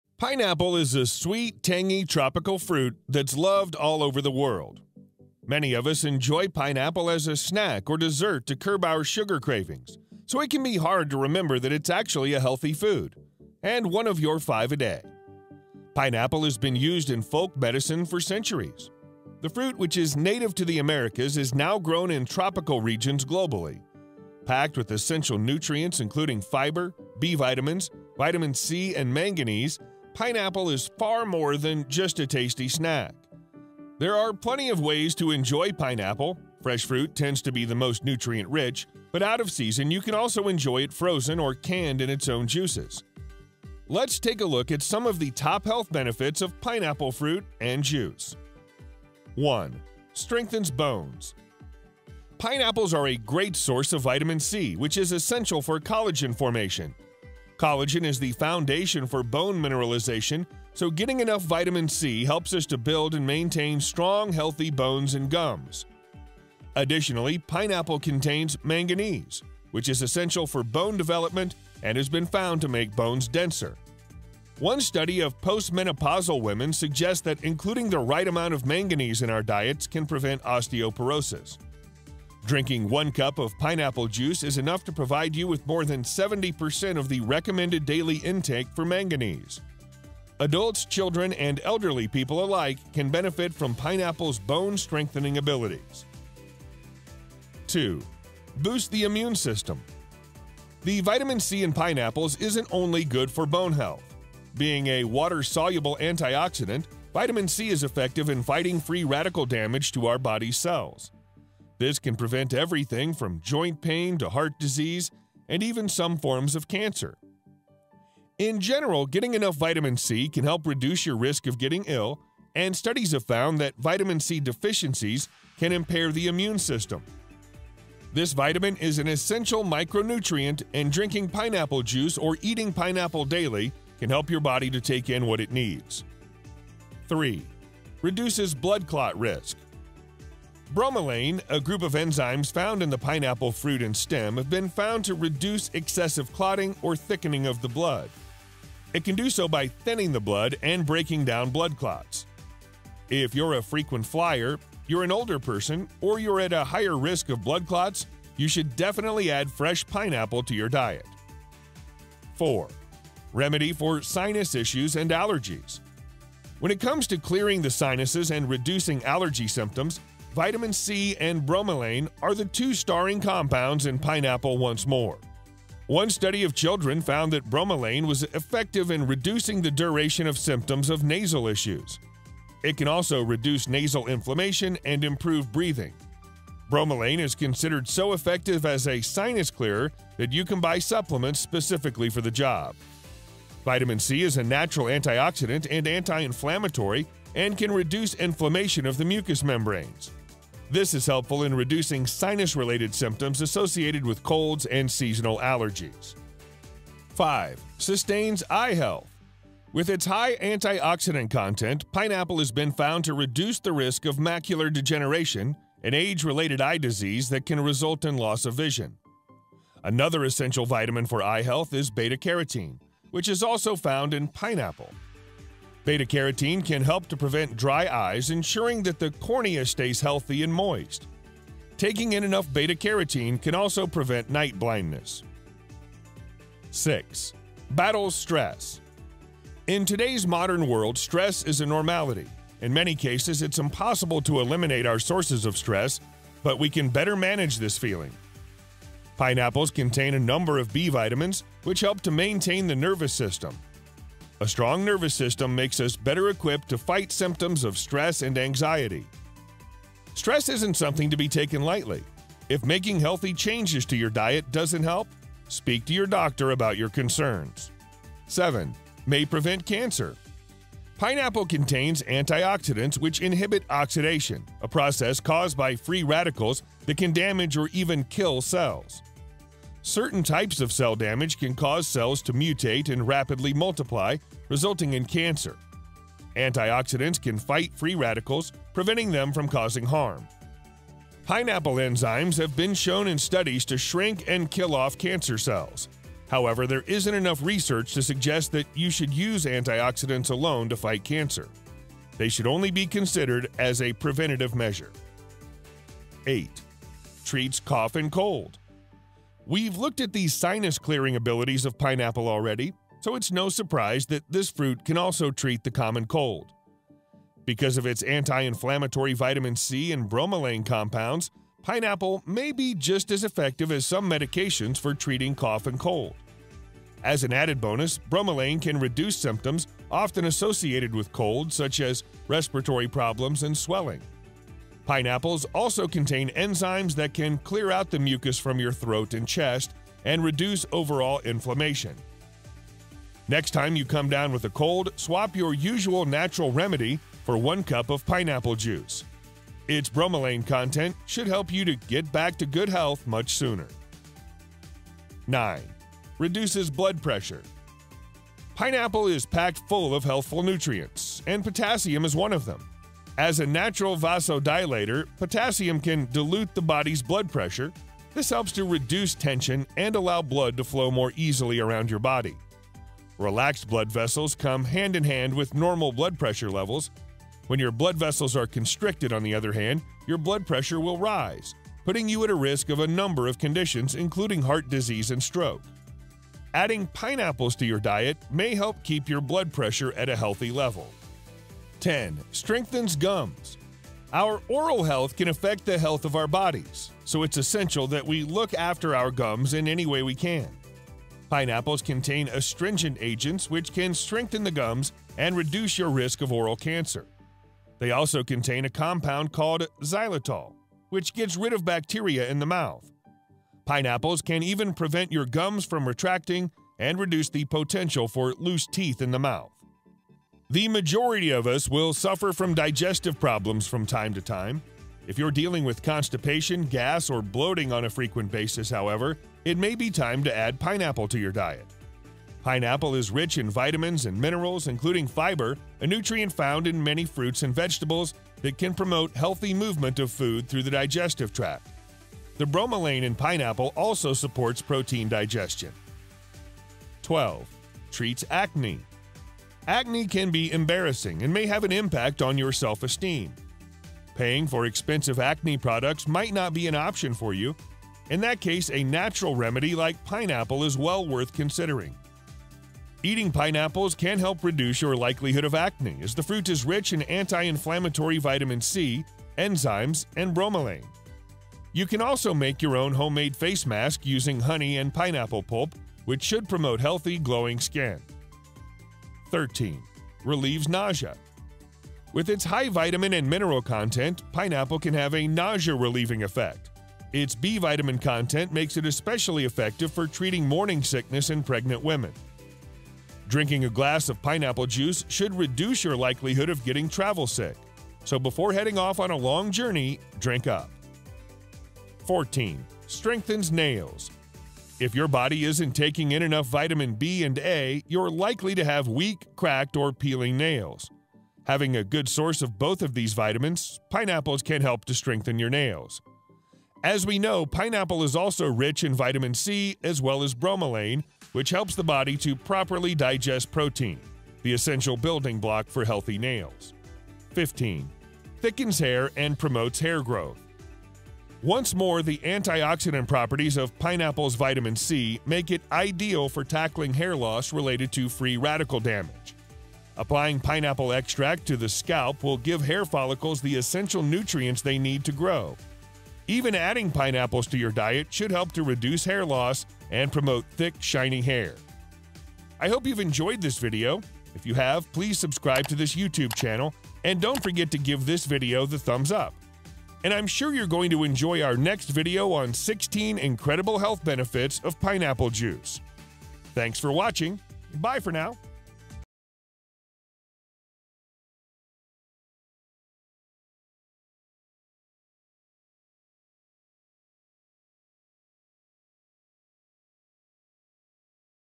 15 SCIENCE BASED Pineapple Health Benefits & Nutrition Facts For the Body
Pineapple is a sweet, tangy tropical fruit that's loved all over the world. Many of us enjoy pineapple as a snack or dessert to curb our sugar cravings, so it can be hard to remember that it's actually a healthy food - and one of your 5 a day!
Pineapple has been used in folk medicine for centuries. The fruit, which is native to the Americas, is now grown in tropical regions globally. Packed with essential nutrients, including fiber, B vitamins, vitamin C and manganese, pineapple is far more than just a tasty snack.
There are plenty of ways to enjoy pineapple. Fresh fruit tends to be the most nutrient-rich, but out of season, you can also enjoy it frozen or canned in its own juices.
Let's take a look at some of the top health benefits of pineapple fruit and juice.
1. Strengthens Bones
Pineapples are a great source of vitamin C, which is essential for collagen formation. Collagen is the foundation for bone mineralization, so getting enough vitamin C helps us to build and maintain strong, healthy bones and gums.
Additionally, pineapple contains manganese, which is essential for bone development and has been found to make bones denser. One study of postmenopausal women suggests that including the right amount of manganese in our diets can prevent osteoporosis.
Drinking one cup of pineapple juice is enough to provide you with more than 70% of the Recommended daily intake for manganese. Adults, children and elderly people alike can benefit from pineapple's bone-strengthening abilities.
2. Boosts The Immune System
The vitamin C in pineapples isn't only good for bone health. Being a water-soluble antioxidant, vitamin C is effective in fighting free radical damage to our body's cells. This can prevent everything from joint pain to heart disease and even some forms of cancer.
In general, getting enough vitamin C can help reduce your risk of getting ill, and studies have found that vitamin C deficiencies can impair the immune system. This vitamin is an essential micronutrient, and drinking pineapple juice or eating pineapple daily can help your body to take in what it needs.
3. Reduces Blood Clot Risk
Bromelain, a group of enzymes found in the pineapple fruit and stem, has been found to reduce excessive clotting or thickening of the blood. It can do so by thinning the blood and breaking down blood clots.
If you're a frequent flyer, you're an older person, or you're at a higher risk of blood clots, you should definitely add fresh pineapple to your diet.
4. Remedy For Sinus Issues and Allergies
When it comes to clearing the sinuses and reducing allergy symptoms, vitamin c and bromelain are the two starring compounds in pineapple once more.
One study of children found that bromelain was effective in reducing the duration of symptoms of nasal issues. It can also reduce nasal inflammation and improve breathing. Bromelain is considered so effective as a sinus clearer that you can buy supplements specifically for the job.
Vitamin C is a natural antioxidant and anti-inflammatory, and can reduce inflammation of the mucus membranes. This is helpful in reducing sinus-related symptoms associated with colds and seasonal allergies.
5. Sustains Eye Health
With its high antioxidant content, pineapple has been found to reduce the risk of macular degeneration, an age-related eye disease that can result in loss of vision.
Another essential vitamin for eye health is beta carotene, which is also found in pineapple. Beta carotene can help to prevent dry eyes, ensuring that the cornea stays healthy and moist. Taking in enough beta carotene can also prevent night blindness.
6. Battles Stress
In today's modern world, stress is a normality. In many cases, it's impossible to eliminate our sources of stress, but we can better manage this feeling.
Pineapples contain a number of B-vitamins, which help to maintain the nervous system. A strong nervous system makes us better equipped to fight symptoms of stress and anxiety.
Stress isn't something to be taken lightly. If making healthy changes to your diet doesn't help, speak to your doctor about your concerns.
7. May Prevent Cancer
Pineapple contains antioxidants, which inhibit oxidation, a process caused by free radicals that can damage or even kill cells.
Certain types of cell damage can cause cells to mutate and rapidly multiply, resulting in cancer. Antioxidants can fight free radicals, preventing them from causing harm.
Pineapple enzymes have been shown in studies to shrink and kill off cancer cells. However, there isn't enough research to suggest that you should use antioxidants alone to fight cancer - they should only be considered as a preventative measure.